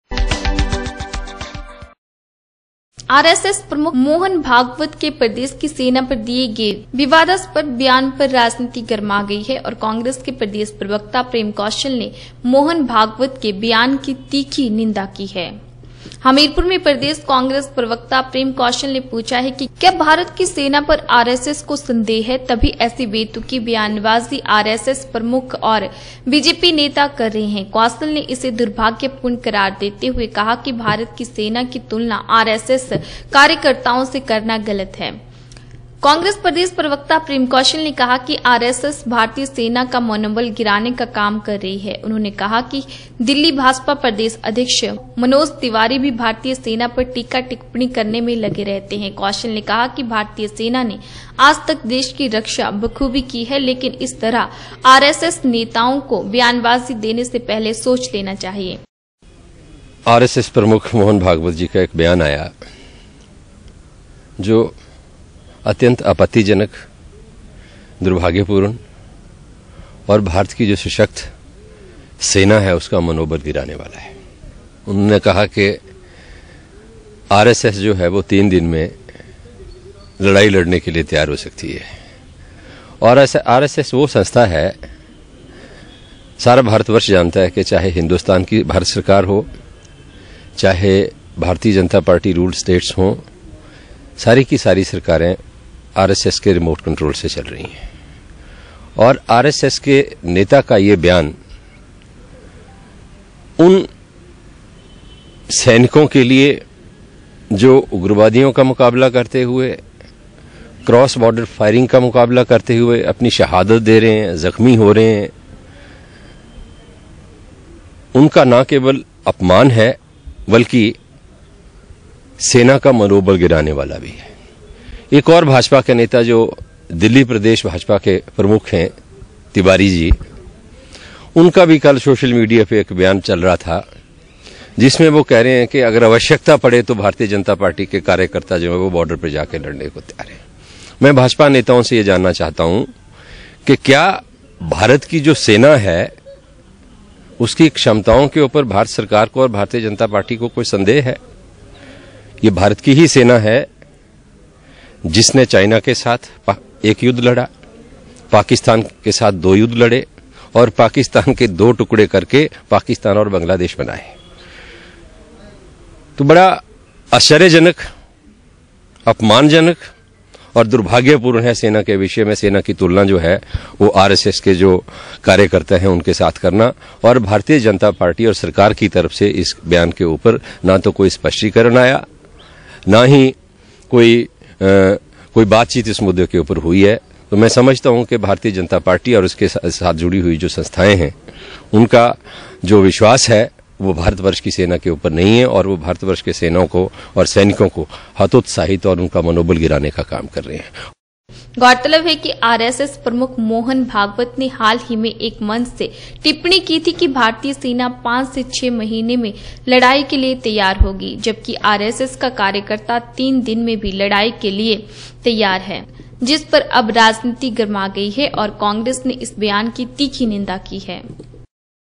आरएसएस प्रमुख मोहन भागवत के देश की सेना पर दिए गए विवादास्मद बयान पर राजनीति गरमा गई
आरएसएस प्रमुख मोहन भागवत के देश की सेना पर दिए गए विवादास्मद बयान पर राजनीति गरमा गई है और कांग्रेस के प्रदेश प्रवक्ता प्रेम कौशल ने मोहन भागव त के ब्यान की तीखी निंदा की है। हमीरपुर में प्रदेश कांग्रेस प्रवक्ता प्रेम कौशल ने पूछा है कि क्या भारत की सेना पर आरएसएस  को संदेश है तभी ऐसी बेतुकी बयानबाजी आरएसएस प्रमुख और बीजेपी नेता कर रहे है। कौशल ने इसे दुर्भाग्र्यपूर्ण  करार देते हुए  कहा कि भारत की सेना की तुलना आरएसएस कार्यकर्ताओंसे करना गलत है।